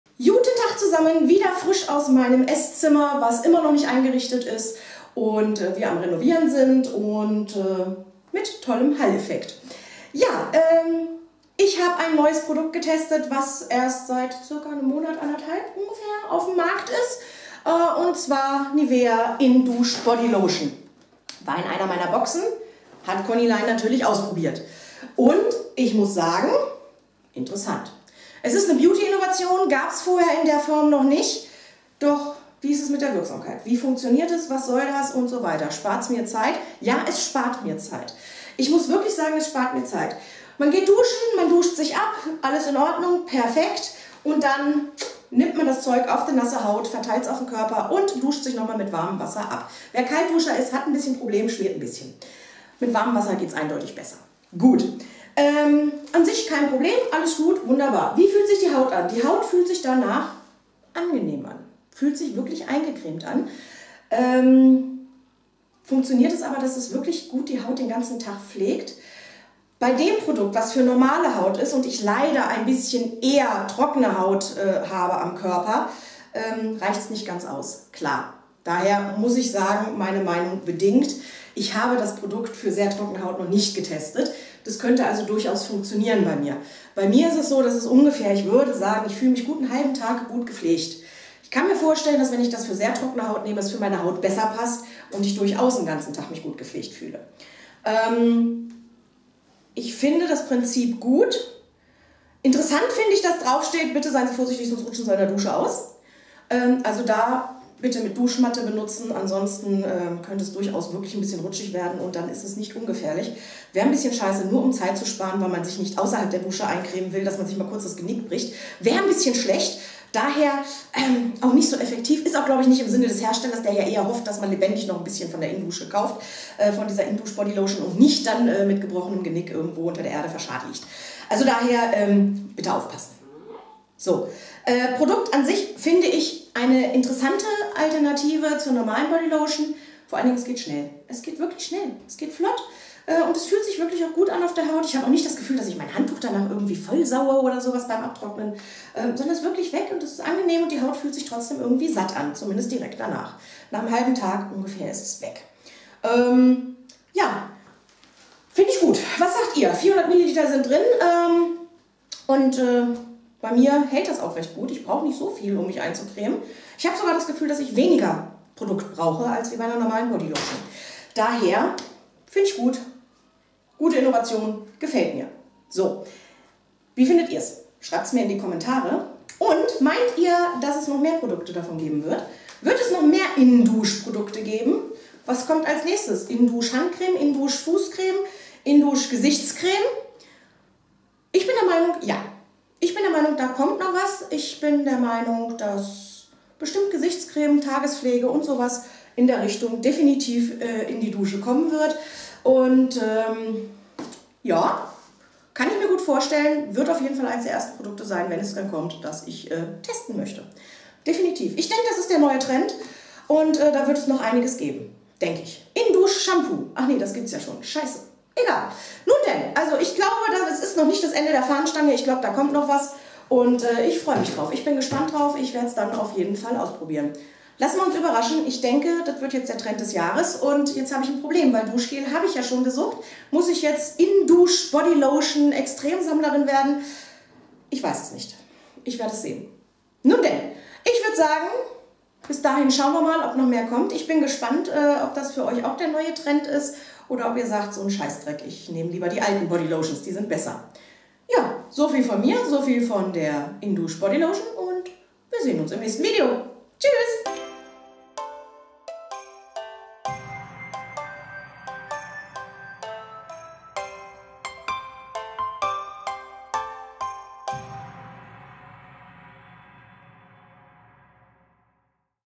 Nivea In-Dusch Bodylotion - der neu innovative Beauty Trend 2013
Ich habe mich mit der Nivea In-Dusch Body Lotion befasst und bin der Meinung, da kommt noch was. Das wird nicht das letzte In-Dusch Produkt sein. Für mich der Beautytrend 2013.
.: Meine Webwelten :.

.: Mein Beautyblog :.

.: Liken :.

.: Freunde sein? :.

.: Gezwitscher :.

.: Google+ :.